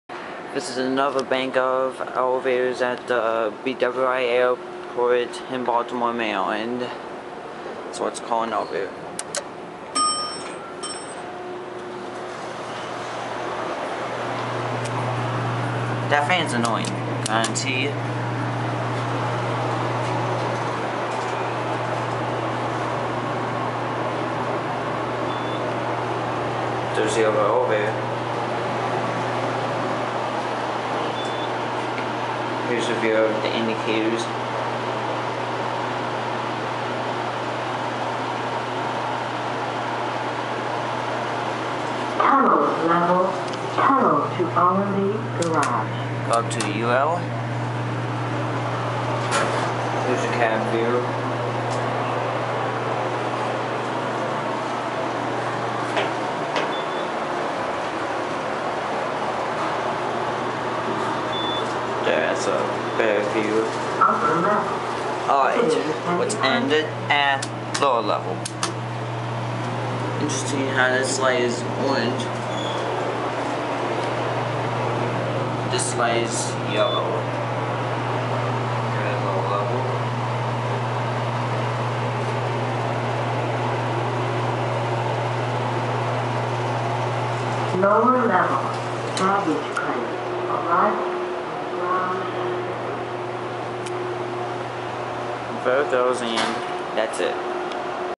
Unknown Hydraulic Elevator at the BWI Marshall Aiport Baltimore MD
Riding some more Unknown Elevators at the BWI Marshall airport.